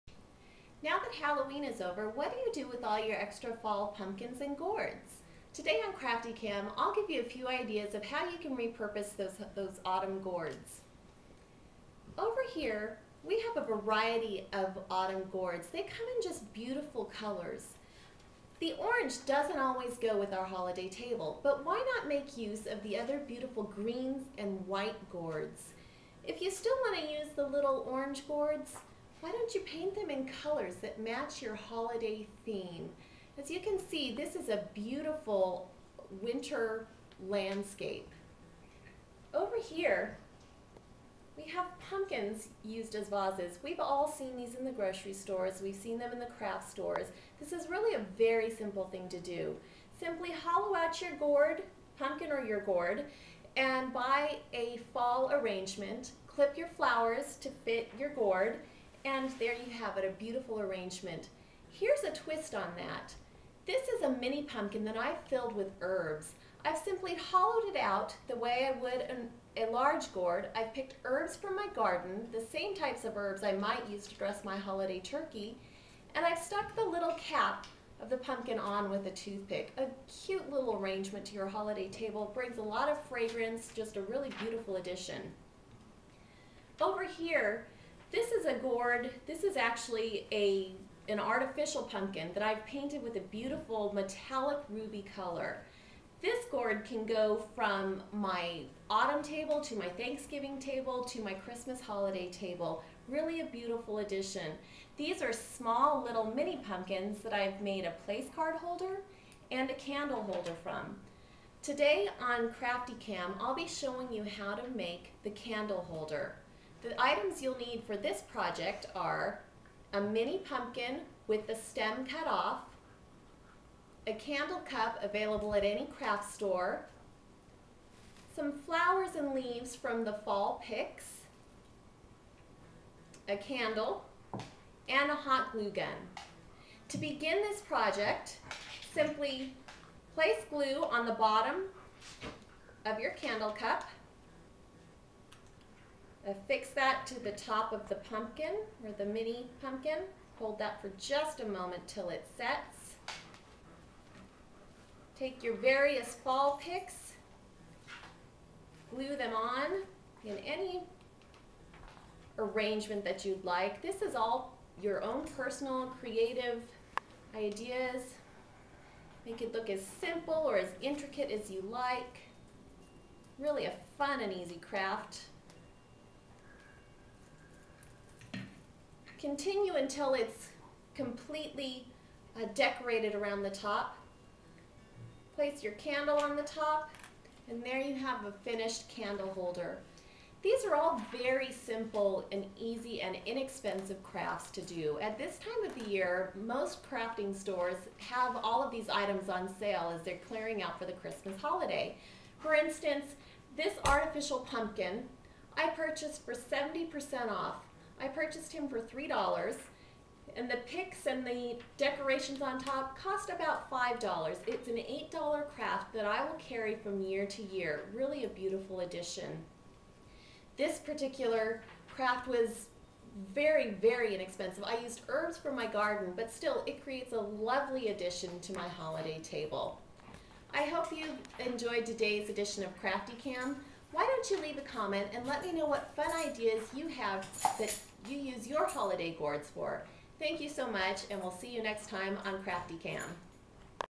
DIY Fall Gourds Crafty Cam
Project ides for re-purposing fall gourds.